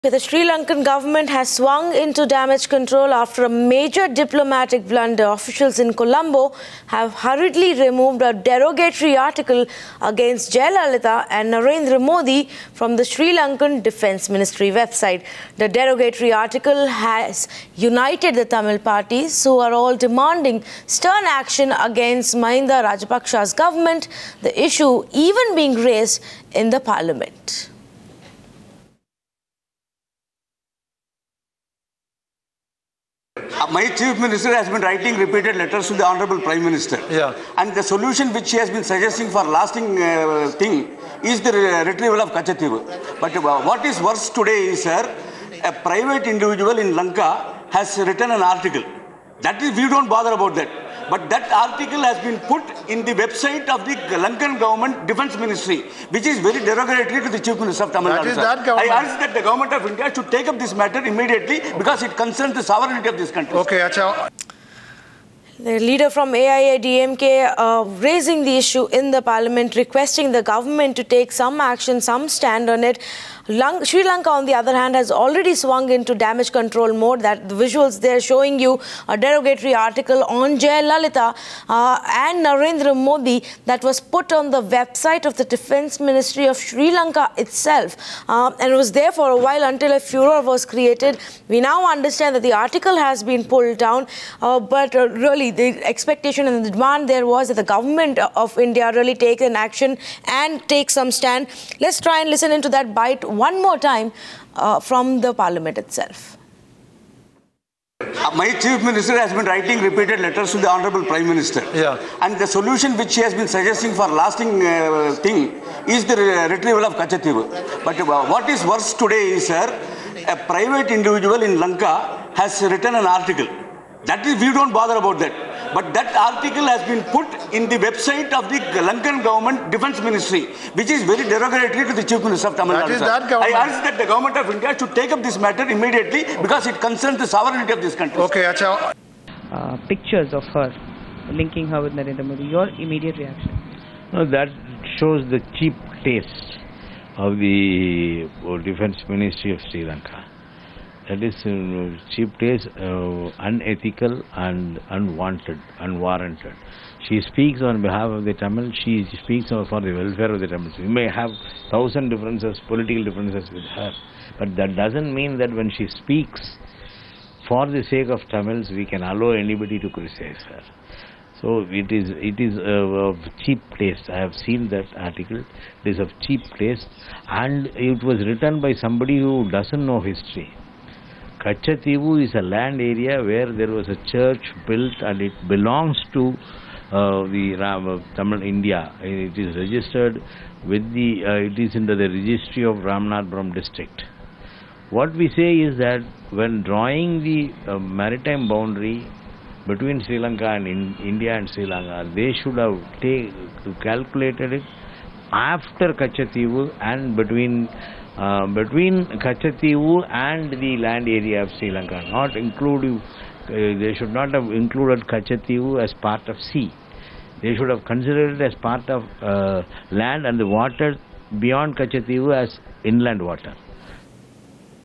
Derogatory article against Jayalalithaa
Sri Lankan Government has swung into damage control after a major diplomatic blunder. Officials in Colombo have hurriedly removed a derogatory article against Tamil Nadu Chief Minister Jayalalithaa and Narendra Modi from the Sri Lankan Defence Ministry website. The derogatory article has united the Tamil parties who are all demanding stern action against Mahinda Rajapaksas government. The issue is even being raised in Parliament. Social Media Links :-